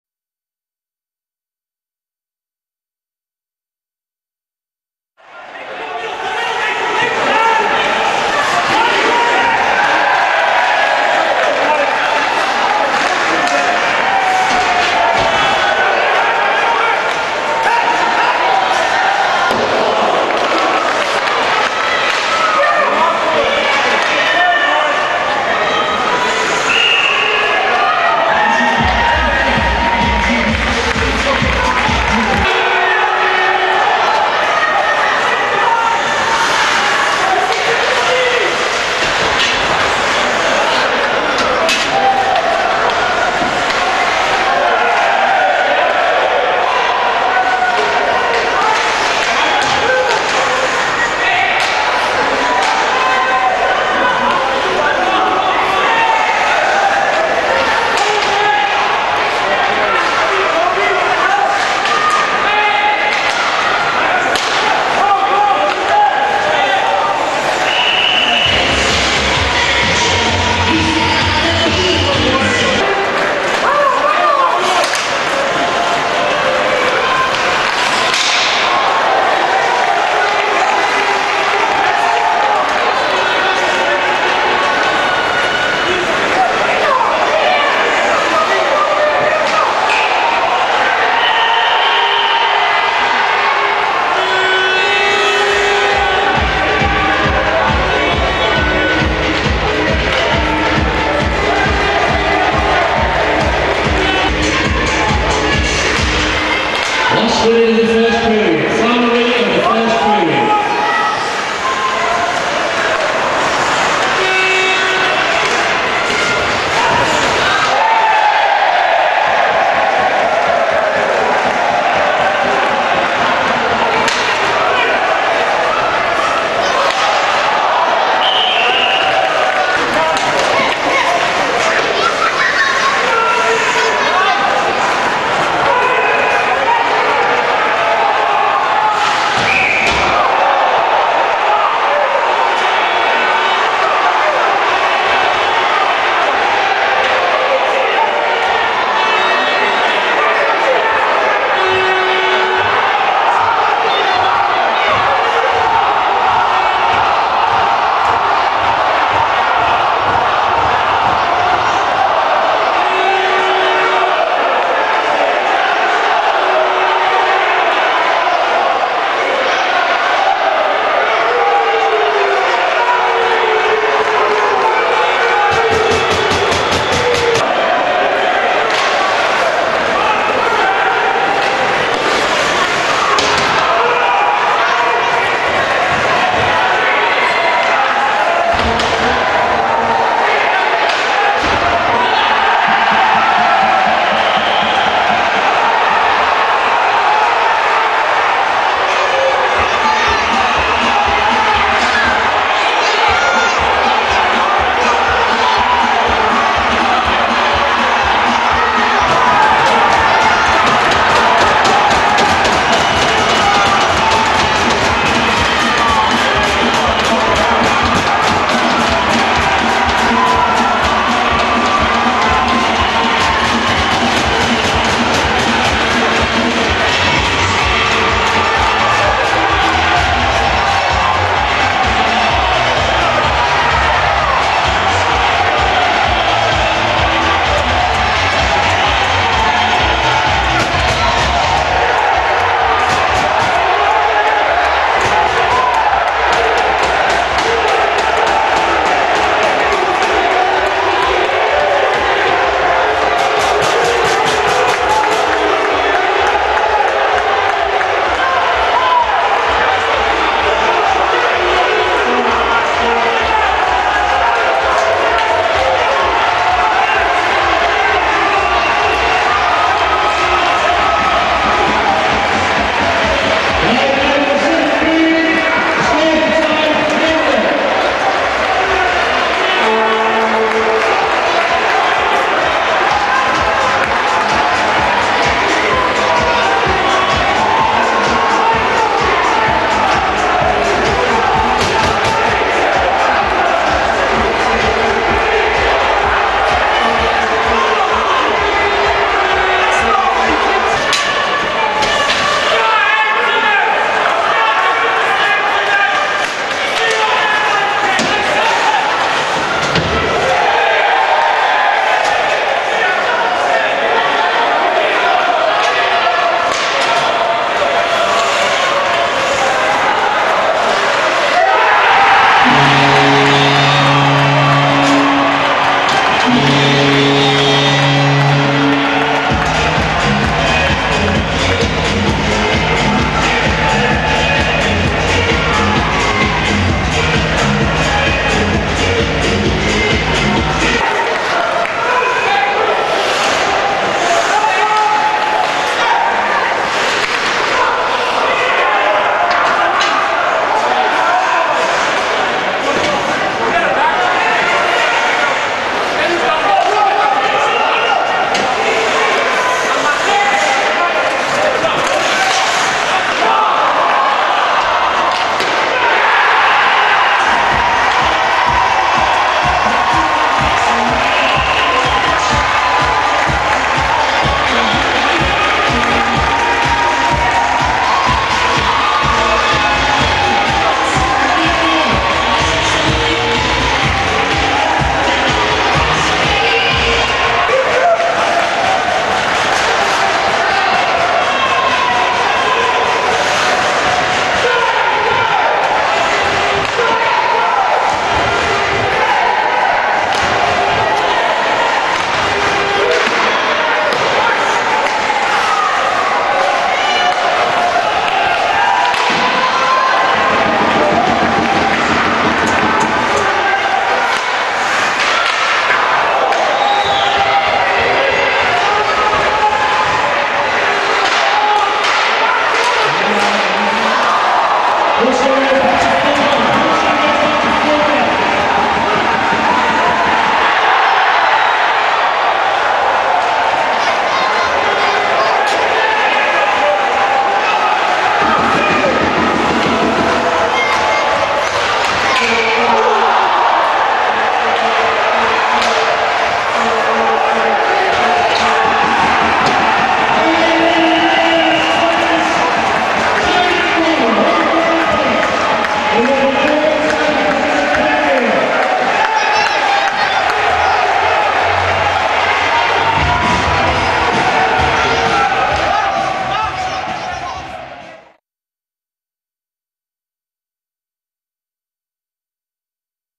Hull Stingrays V Nottingham Panthers 28-11-2010
Hull Stingrays V Nottingham Panthers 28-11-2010 Elite League Challenge Cup Highlights